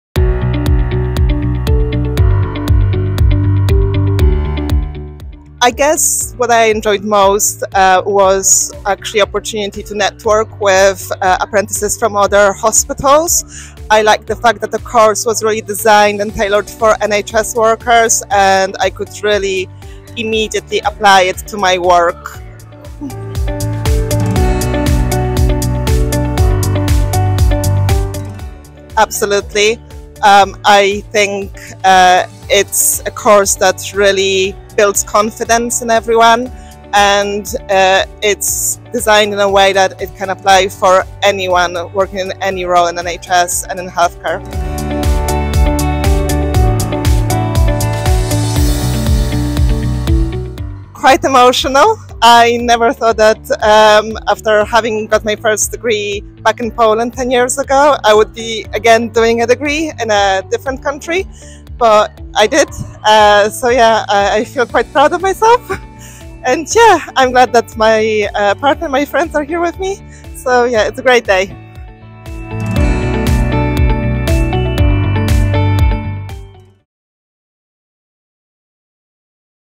Chartered Manager in Health and Social Care Graduate - Class of 2024
Meet our Chartered Manager in Health and Social Care degree apprentice graduate, Gosia Walczak, as she shares more about the benefits of the programme to her career and the impact it had on her organisation, the Mid and South Essex NHS Foundation Trust.